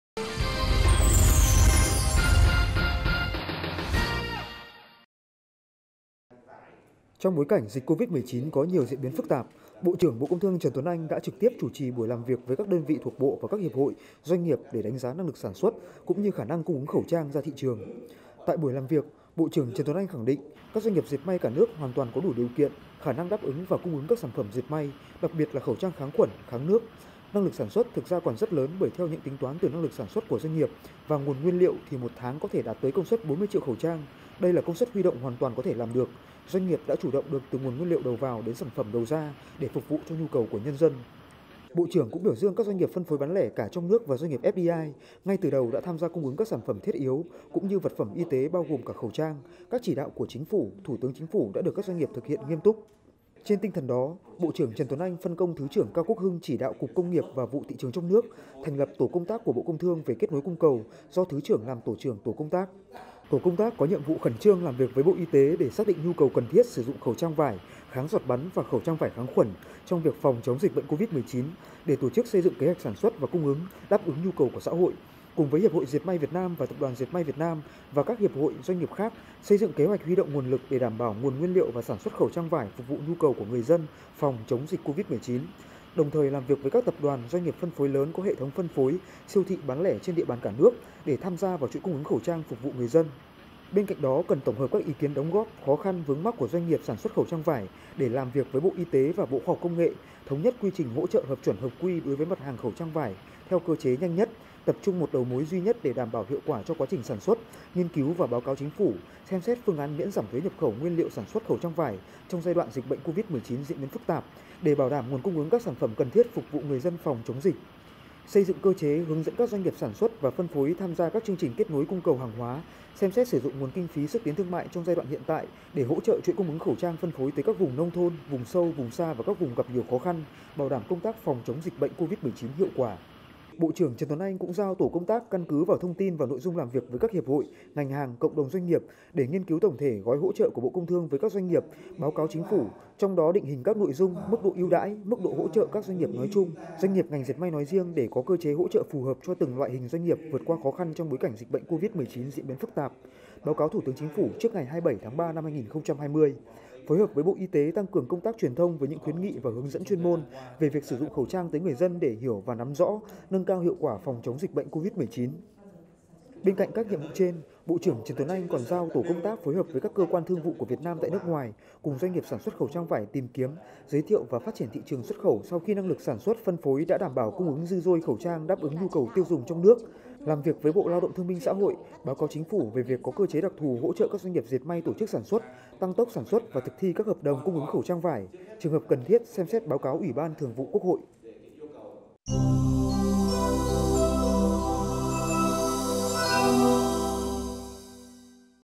Bộ Công Thương thành lập Tổ công tác kết nối cung cầu để điều tiết thị trường khẩu trang
Trong bối cảnh dịch Covid 19 có nhiều diễn biến phức tạp, Bộ trưởng Bộ Công Thương Trần Tuấn Anh đã trực tiếp chủ trì buổi làm việc với các đơn vị thuộc Bộ và các Hiệp hội, doanh nghiệp để đánh giá năng lực sản xuất cũng như khả năng cung ứng khẩu trang ra thị trường…